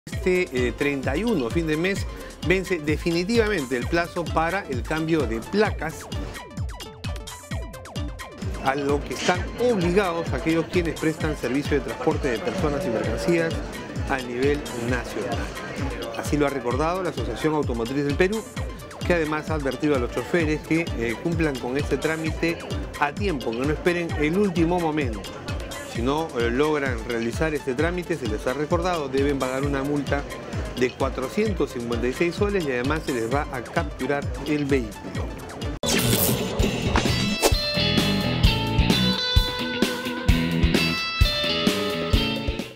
¿Cuándo será el vencimiento del plazo para cambiar las placas?
El próximo 31 de agosto terminará el plazo que dio el Ministerio de Transporte y Comunicaciones para el cambio de las placas para vehículos de trasporte público y servicios. Twitter: @RPPNoticias